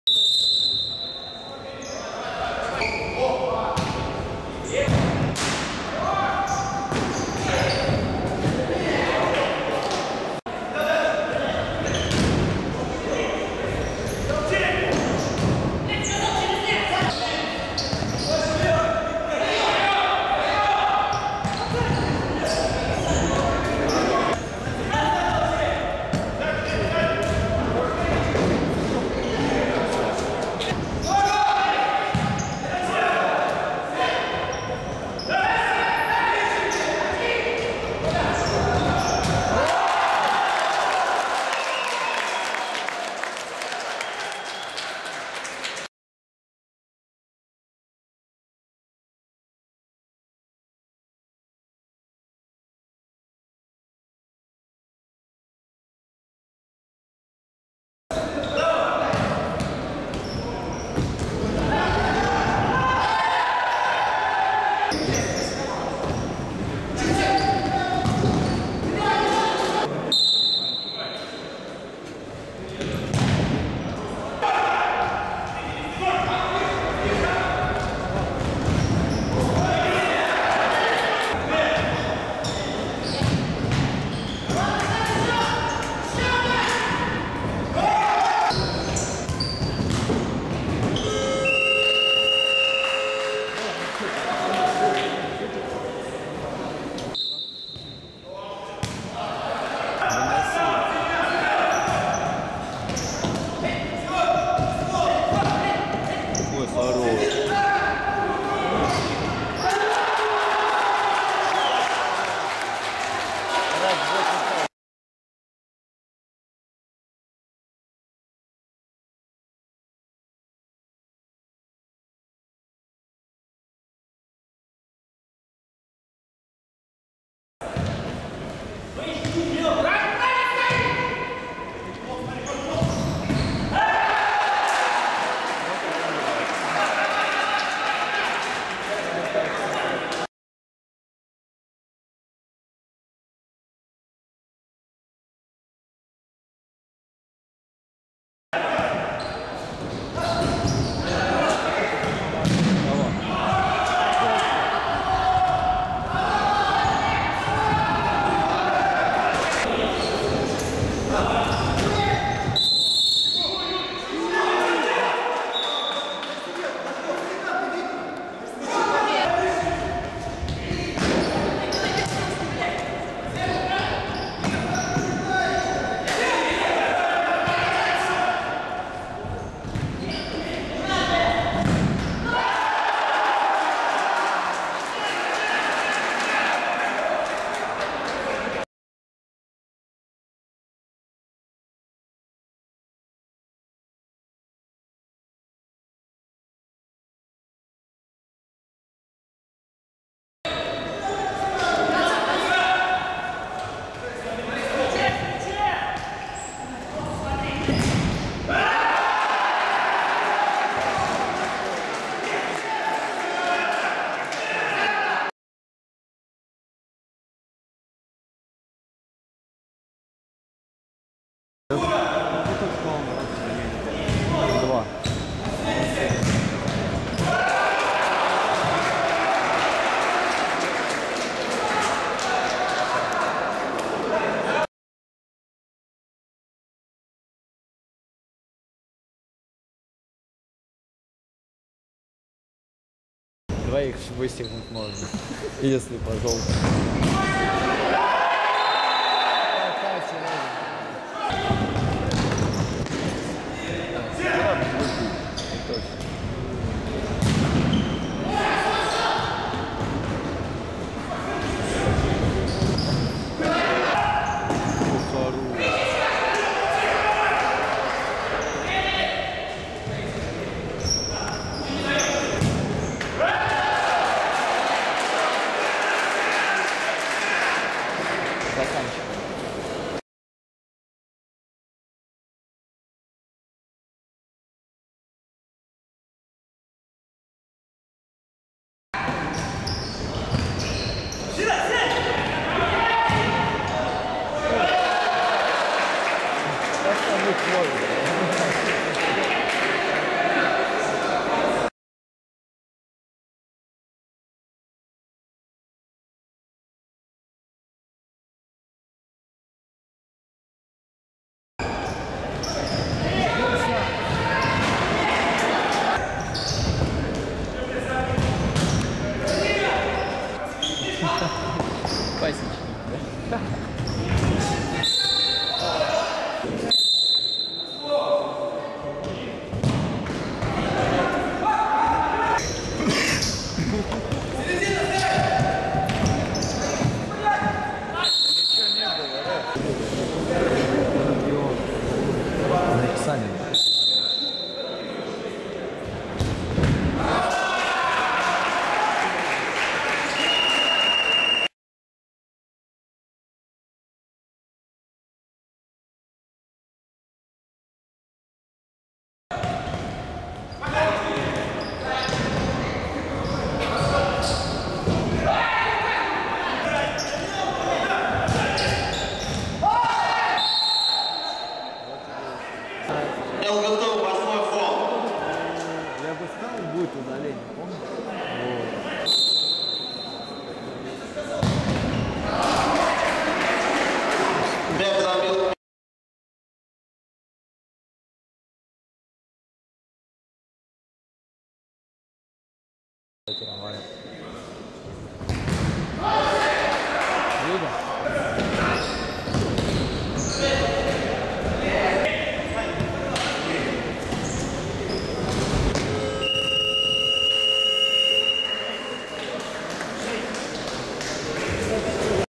Futsal. МФК Липецк - ЛГТУ - 8:2. Обзор
19 марта 2016 года. МФК Липецк - ЛГТУ (Липецк) - 8:2 (0:1). Голы: Воронин, 13:42 (0:1). П.Назаров, 26:17 (0:2). Кондратьев, 28:05 (1:2). Санин, 31:53 (2:2). Толчеев, 33:11 (3:2). Фигурчук, 34:08 (4:2). Логунов, 36:29 (5:2). Назаров, 39:56 (6:2 автогол). Сухарев, 43:59 (7:2 с 10-м). Сухарев, 46:52 (8:2 с 10-м).